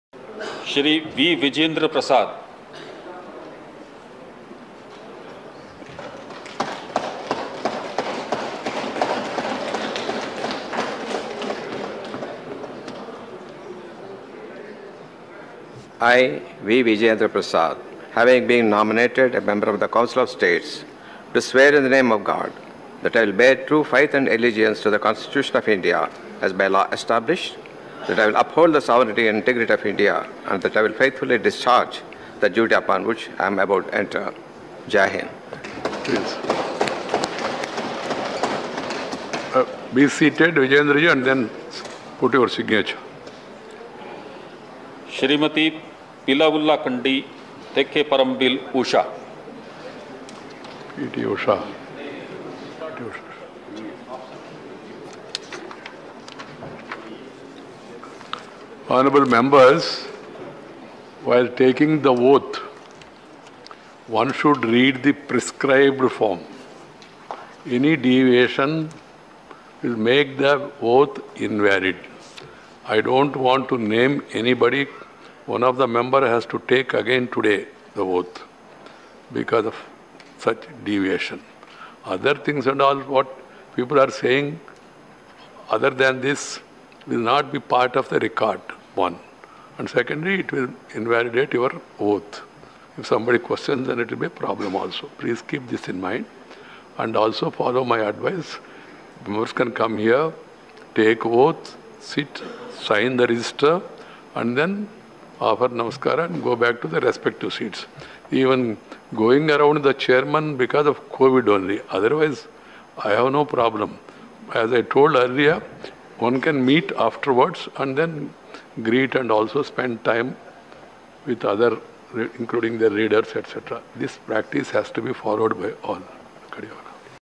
Vijayendra Prasad Takes Oath as Rajya Sabha MP | Writer Vijayendra Prasad | Andhra Life TV
watch here : Vijayendra Prasad Takes Oath as Rajya Sabha MP | Writer Vijayendra Prasad | Andhra Life TV